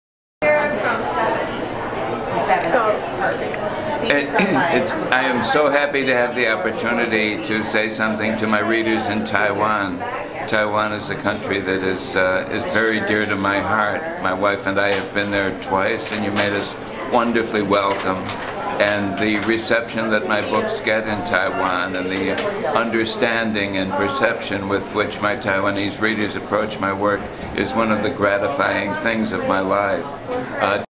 卜老給台灣讀者的問安喔!
可愛的卜老又來囉~這次他透過鏡頭來跟大家say Hello!
感謝當代劇場的提供,讓台灣的讀者有機會看看卜老可愛的身影阿 :D